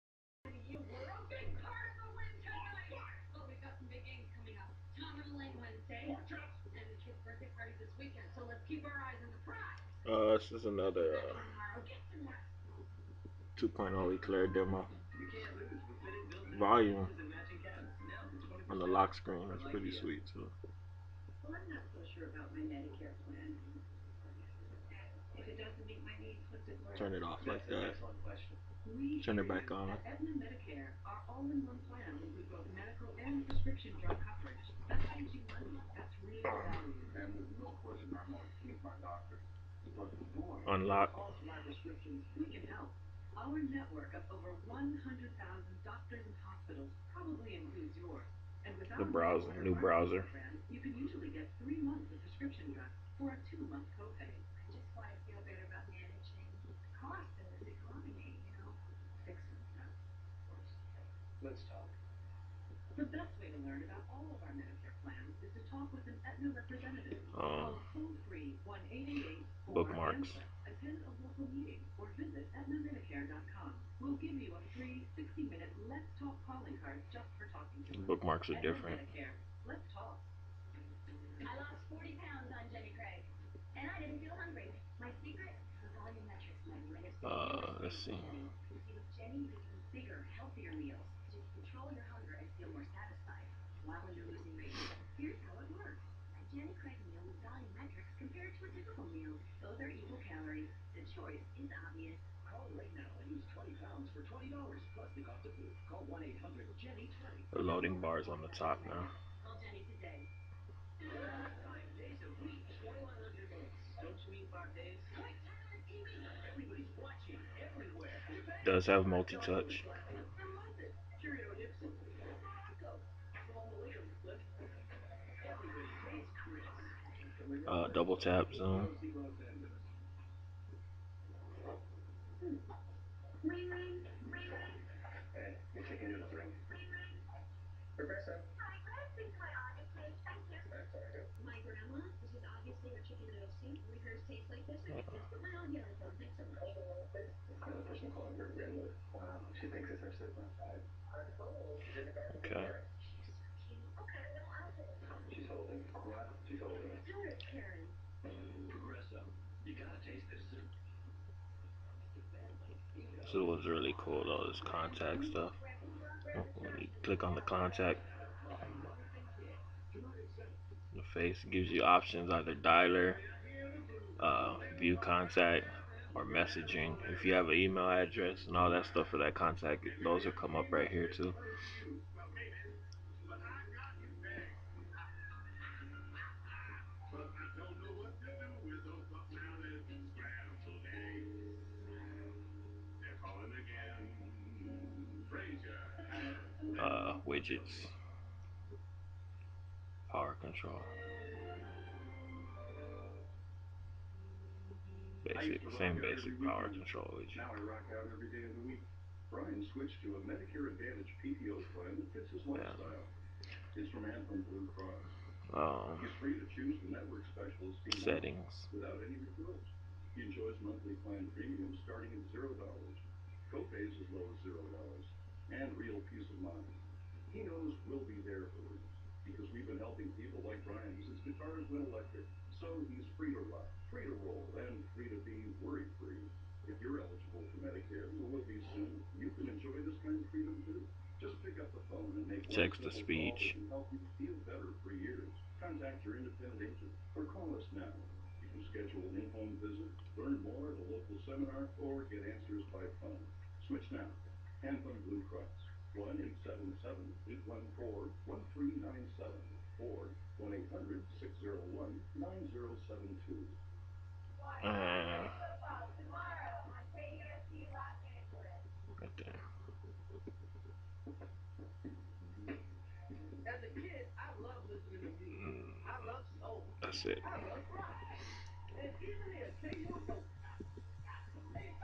Android 2.0 Demo
Eclair demo. Very sweet but not for daily use. (Unstable) No Google apps or Contact sync yet.

Thanks Akirah.